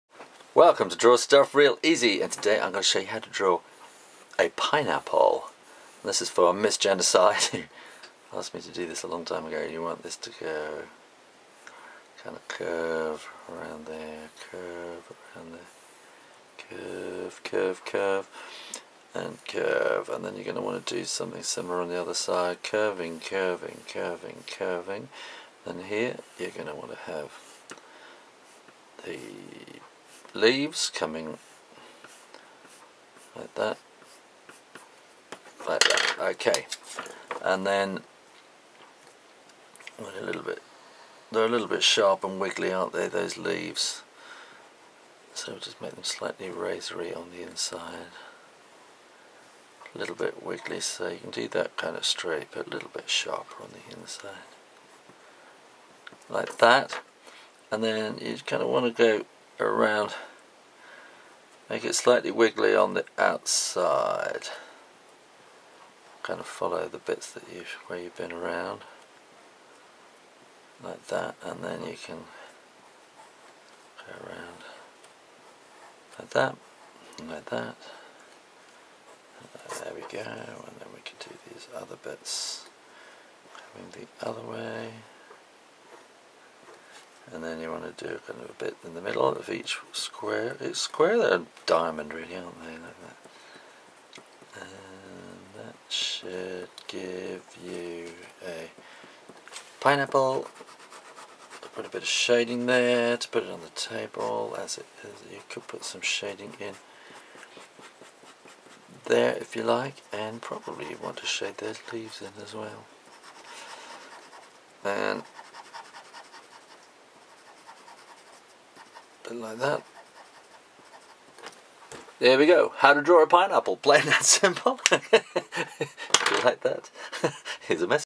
w to draw a pineapple real easy | Step by Step with Easy - Spoken Instructions
Learn how to draw a pineapple real easy | Step by Step with Easy - Spoken Instructions with Shoo Rayner, the author of Everyone Can Draw - the book that teaches you how to draw Real Easy!